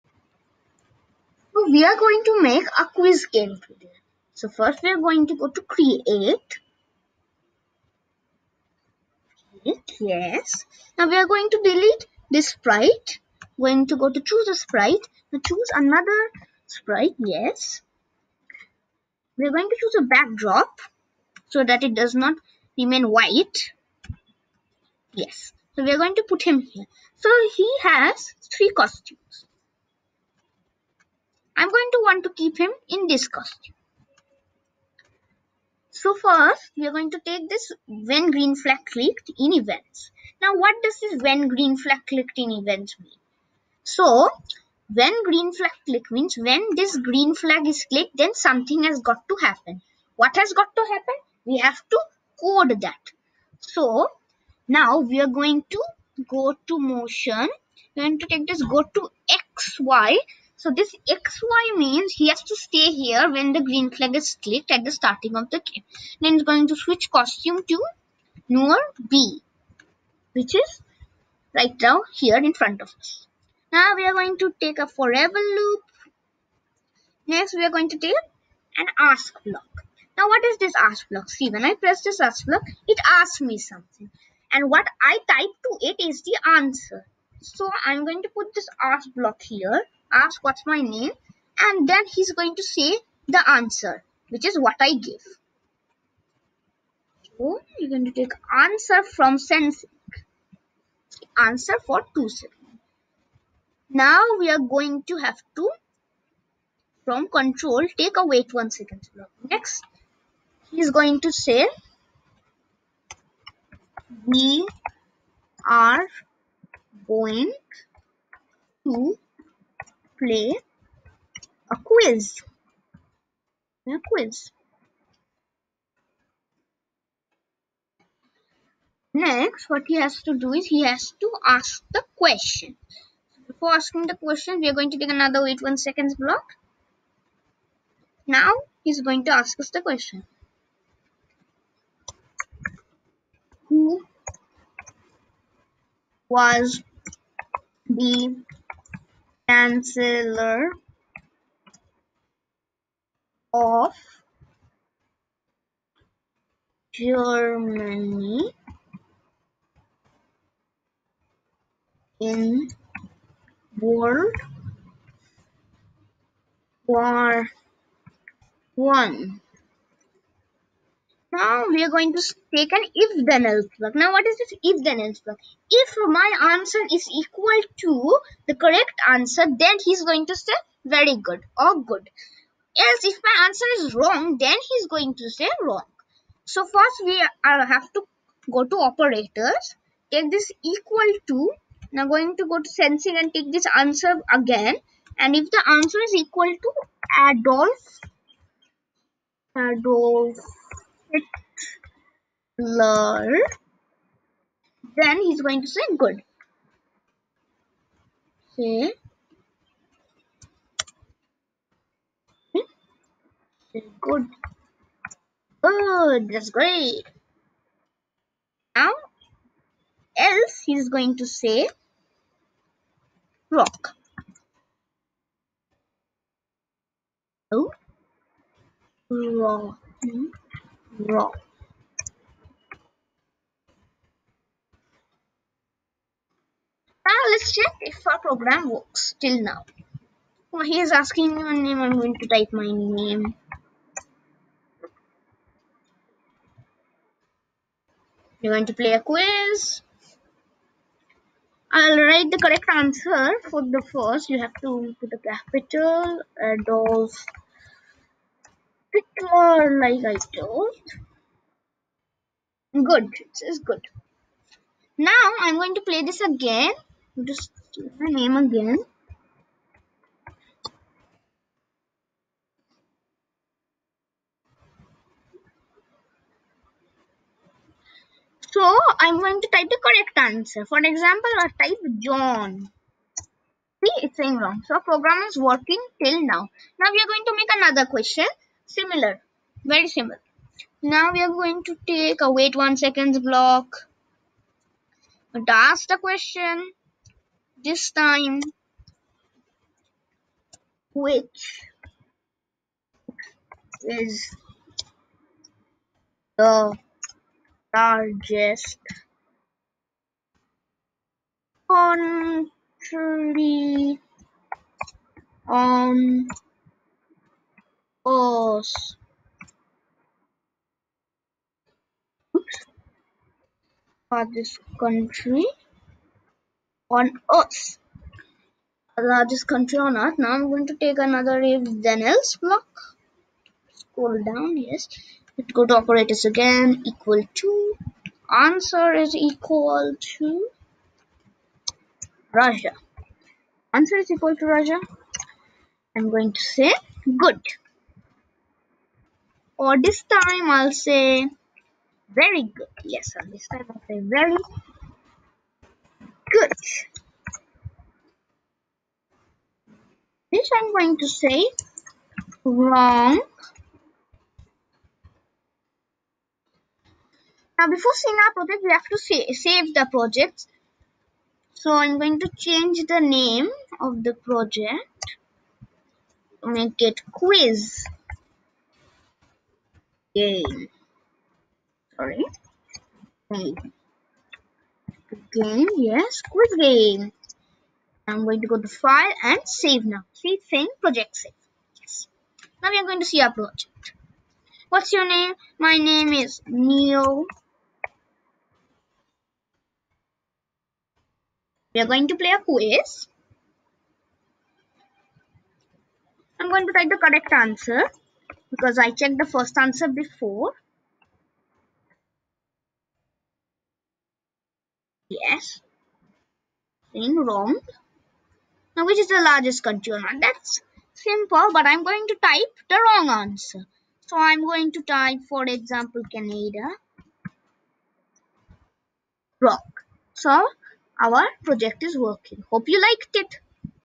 Quiz Game in Scratch tutorial...by a kid for the kids
Get tips on how to build a quiz game